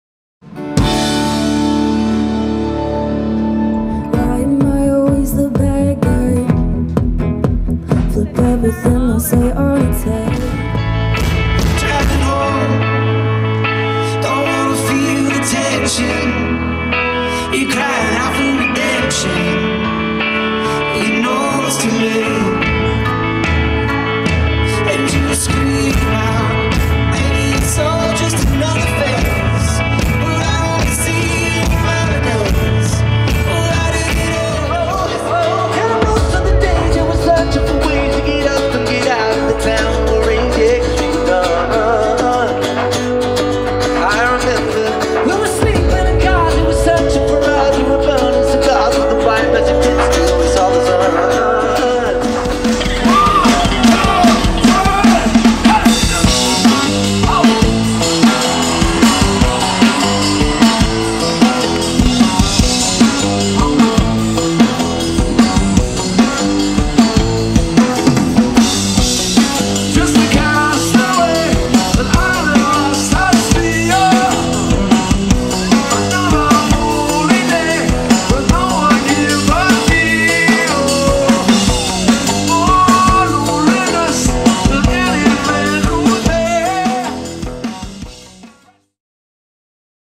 JAS Labor Day Experience: Saturday
HIghlights from Day 2 of the JAS Labor Day Experience on Saturday, September 1, 2024